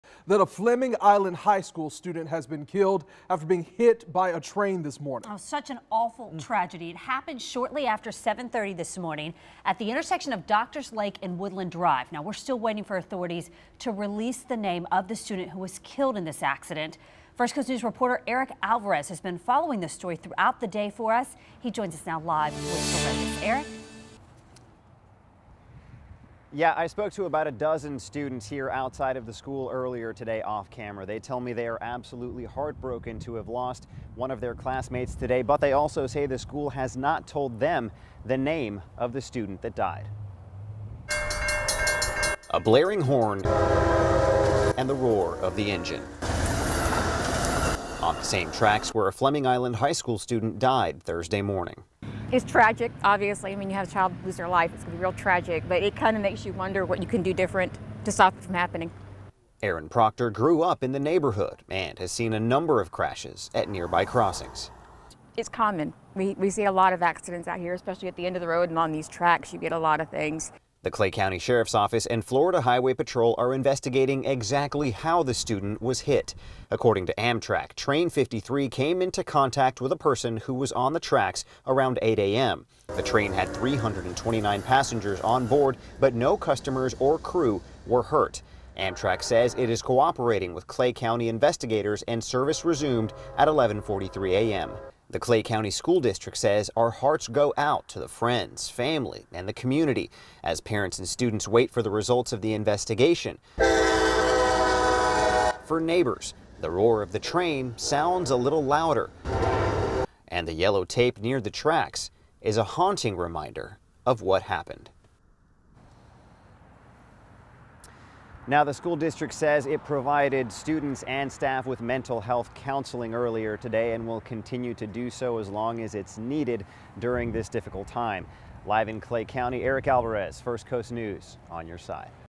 Fleming Island HS student killed by train
It happened shortly after 7:30 this morning at the intersection of Doctor's Lake and Woodland Drive.